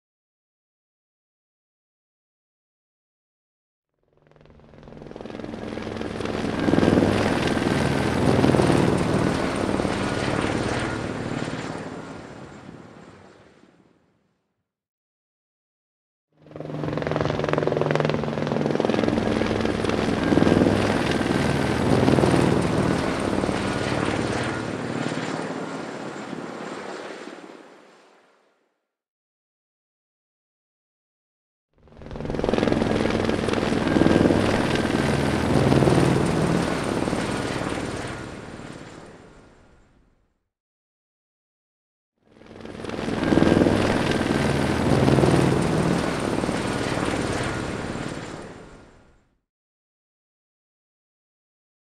Helicopter Flying Over Sound
We recorded a Sound Effect of a Passing Helicopter.
LICENSE AGREEMENT:
► You can’t sell any of the sound effects in their original form for commercial gain and/or claim ownership.
Hardware used to record the sounds:
Zoom H1n
Zoom H4n
Audio-Technica AT875R
Audio-Technica M50X
Free Sound Effects.